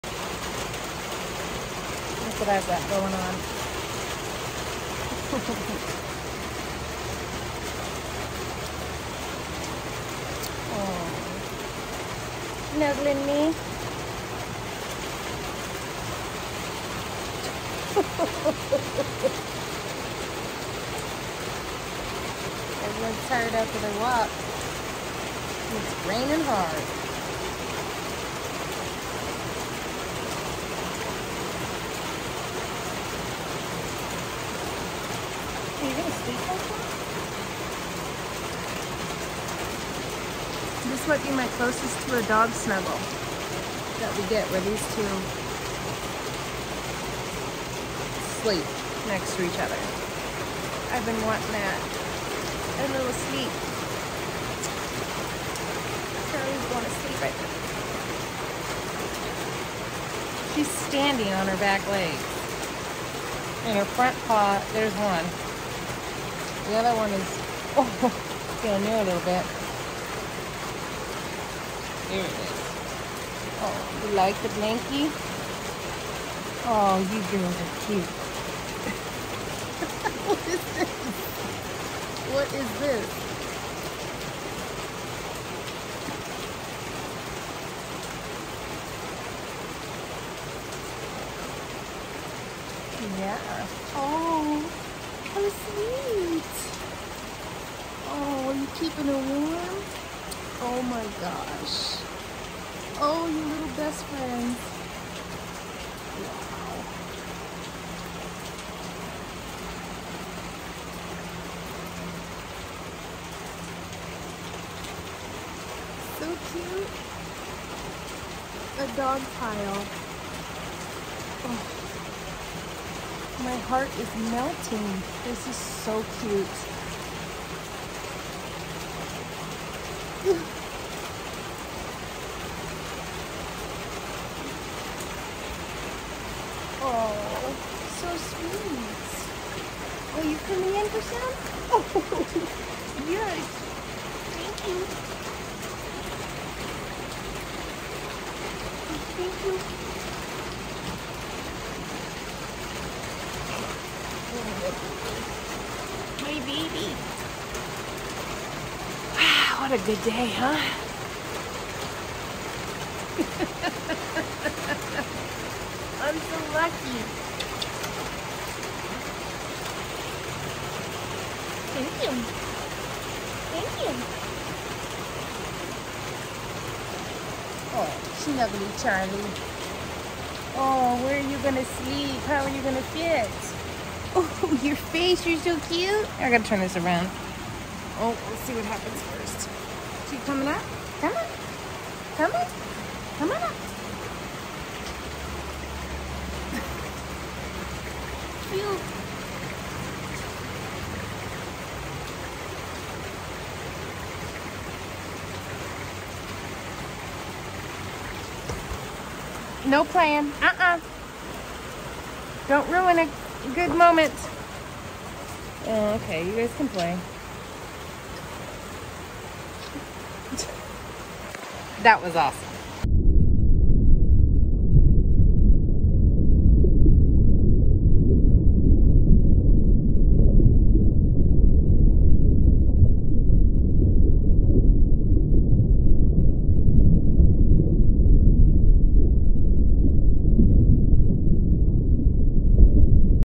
When Dog rest her head on best friend, My heart melted
Puddy and Charlie the Golden Retriever are best friends